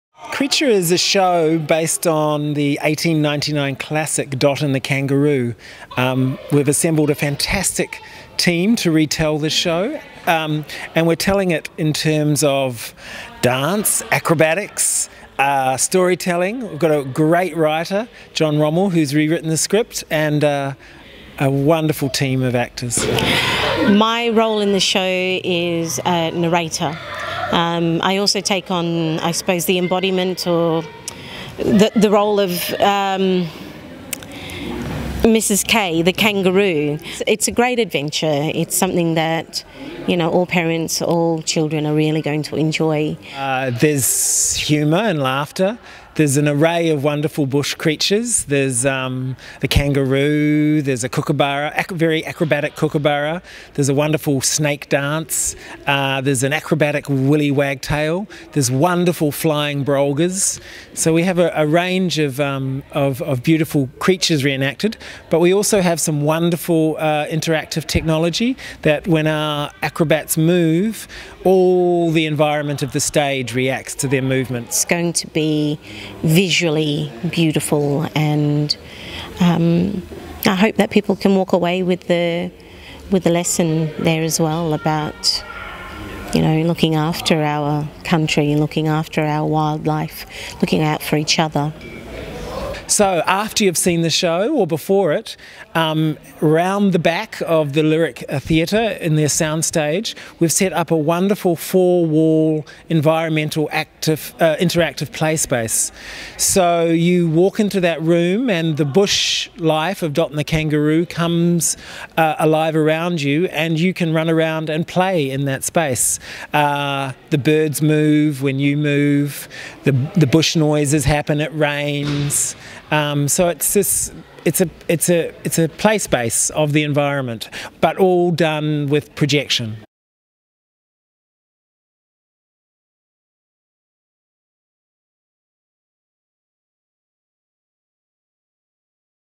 Behind the Scenes of Creature: An Adaptation of Dot and the Kangaroo - Out of the Box at QPAC
Director David Clarkson and performer Ursula Yovich take you behind the scenes of Creature; a new theatre work for children, based on the classic Australian story Dot and The Kangaroo.